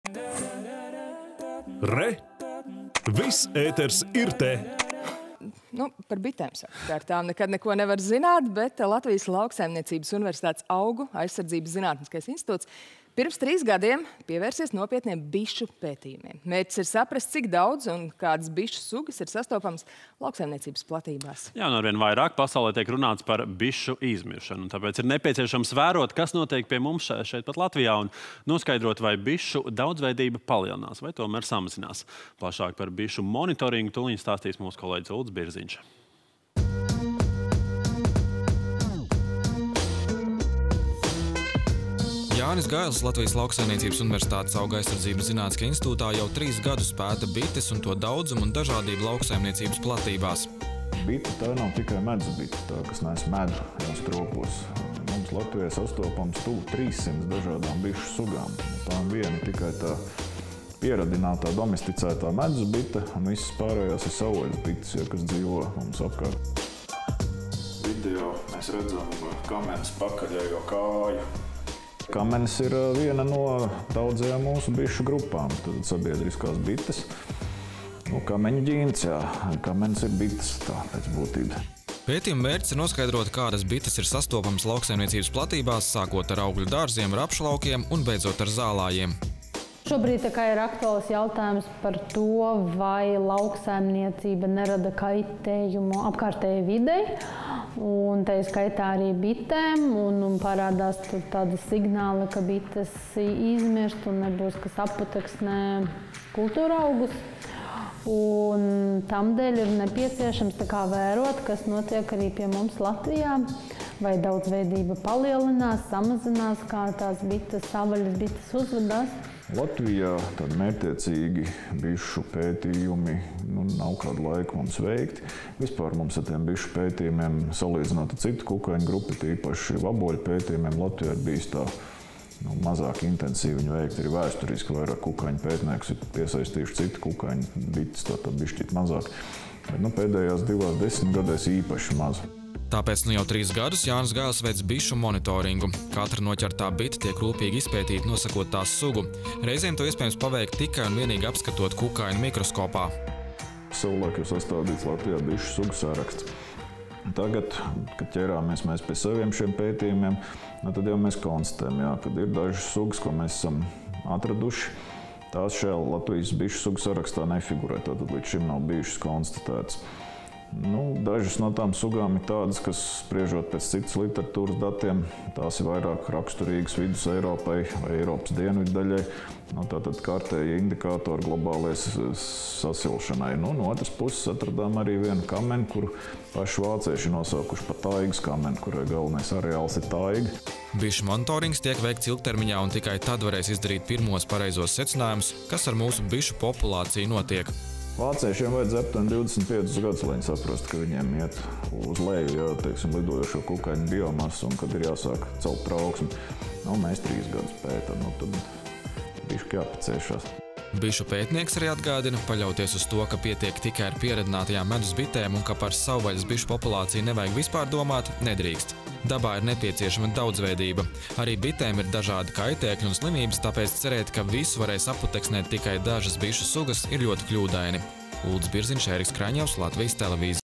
Veic bišu monitoringu
Lai gan ar bitēm nekad neko nevar zināt, Latvijas Lauksaimniecības universitātes Augu aizsardzības zinātniskais institūts pirms trīs gadiem pievērsies nopietniem bišu pētījumiem. Mērķis ir saprast, cik daudz un kādas bišu sugas sastopamas lauksaimniecības platībās. Arvien vairāk pasaulē tiek runāts par bišu izmiršanu. Tāpēc ir nepieciešams vērot, kas notiek pie mums Latvijā, un noskaidrot vai bišu daudzveidība palielinās, vai tomēr samazinās. Plašāk par bišu monitoringu tūliņ pastāstīs kolēģis Uldis Birziņš.